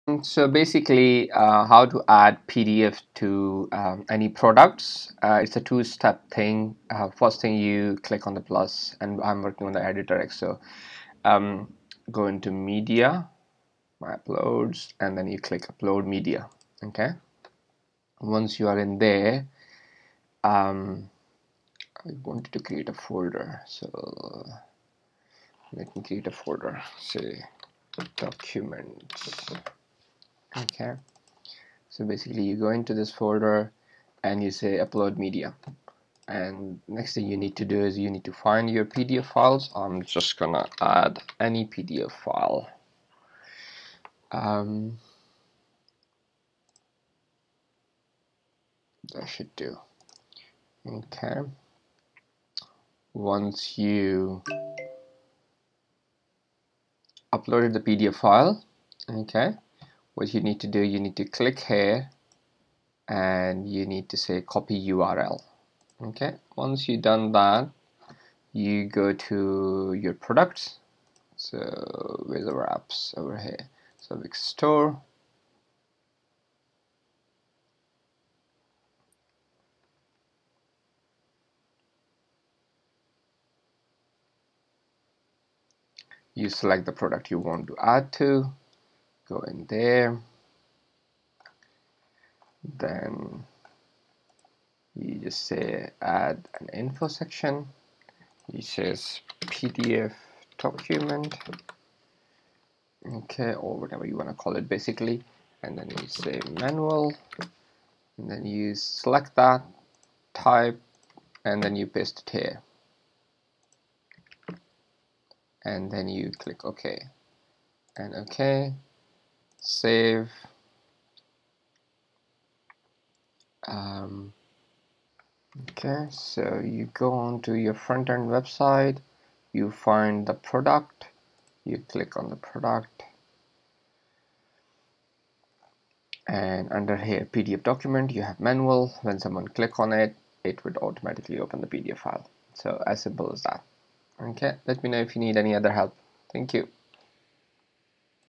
Wix Tutorial: How to attach a pdf/document file to Wix Store Products
Here is a simple quick instructions on how to attach any document file with products on Wix stores.